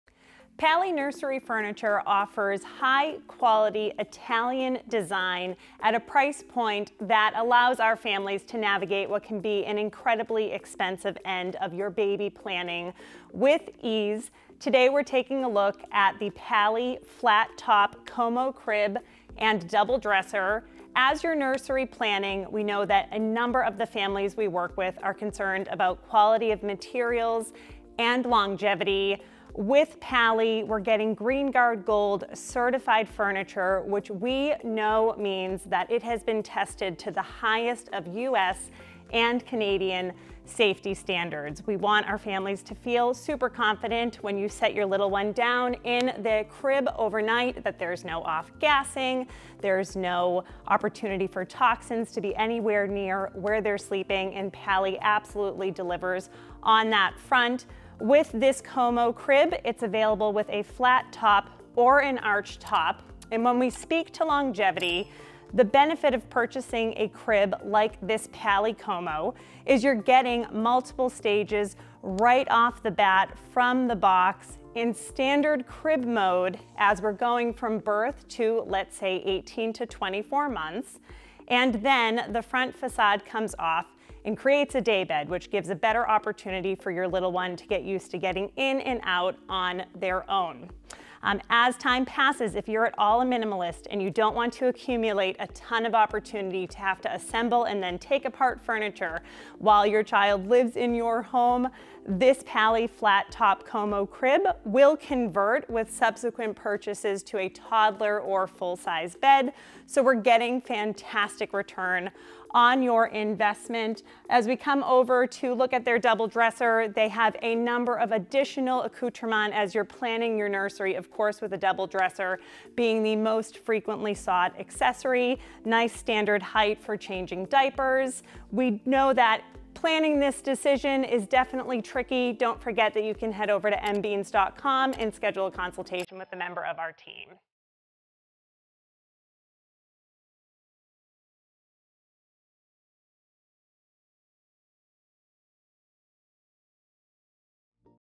Pali Como Crib+ Dresser Review | Nursery Furniture | Best Baby Gear 2022 | Magic Beans Reviews
In this video, Mia reviews the Pali Como Crib and Dresser! The Pali Como Crib and Dresser give you a great bang for your buck, with lots of the features you'd expect from luxury nursery furniture at a more reasonable cost. The hardwood furniture is GreenGuard Gold certified so you can be sure that your baby isn't coming into contact with any toxins or hazardous materials when you lay them down to sleep at night. The crib comes with either a flat or arched top so it can match any nursery aesthetic, and grows with your child from infant to toddler and beyond!